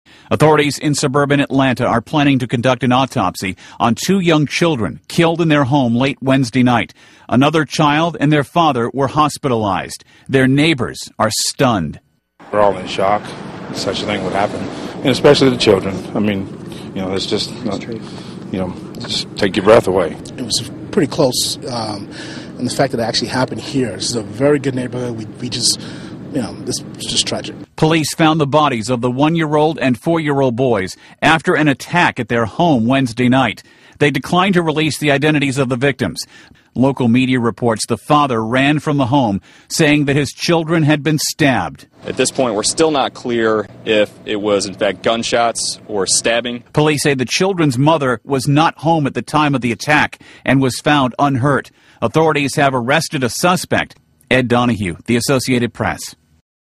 Suspect Arrested, 2 Boys Killed in Family Attack
Police say they have arrested a suspect in a suburban Atlanta attack that left two young boys dead in their home and two other family members injured. They have not released a motive in the attack. (Feb. 11)